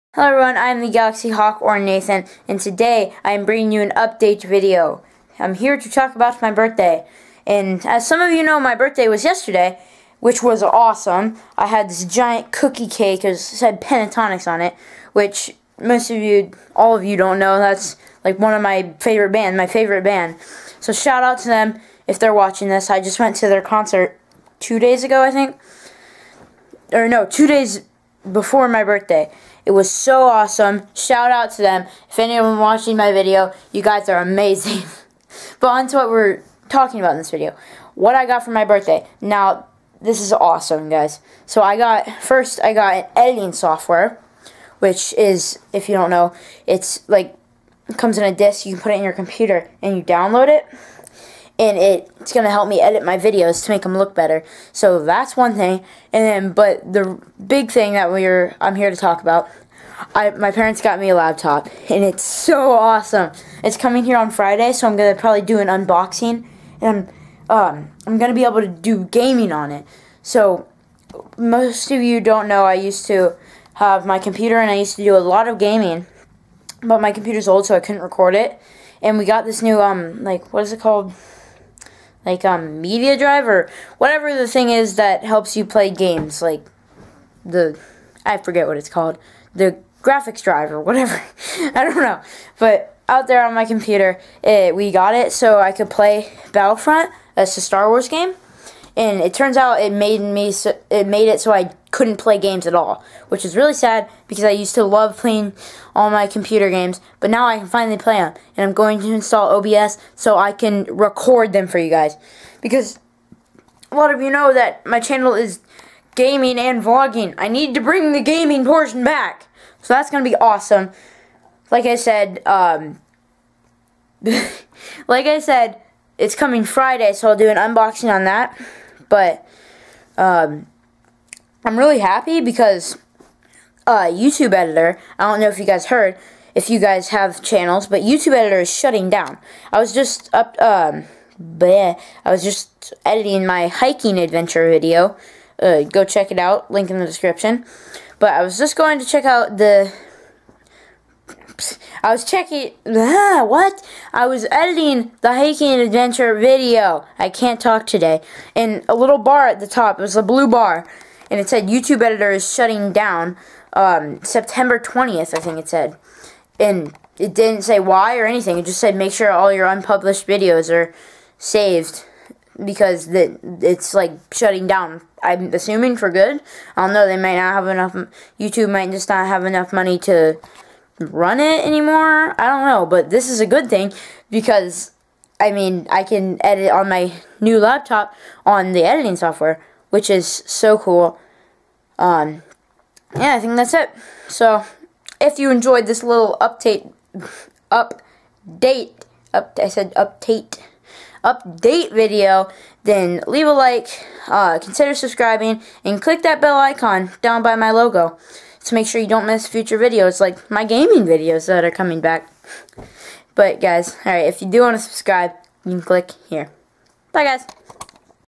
Birthday Update Vlog!
I talk to you about my birthday.